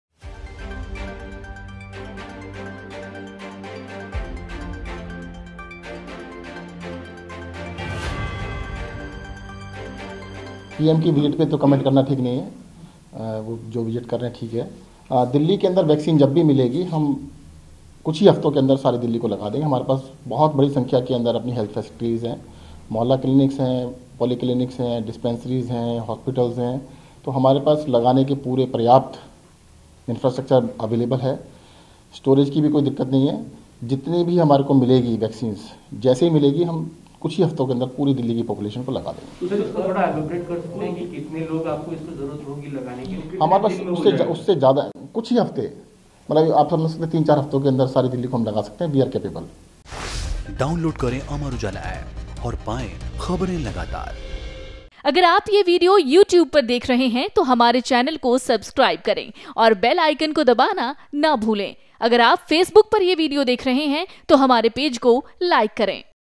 Vaccine पर बोले स्वास्थ्य मंत्री Satyendra Jain- 'कुछ हफ्तों में ही लगा देंगे Corona का टीका'
Delhi में Corona के बढ़ते प्रकोप के बीच सभी को जल्द से जल्द Vaccine के आने का इंतजार है। इसी बीच शनिवार को स्वास्थ्य मंत्री Satyendra Jain ने कहा कि जब भी Delhi में Vaccine आ जाएगी, उसके बाद तीन से चार सप्ताह के भीतर हम राजधानी में उसका वितरण करवा लेंगे।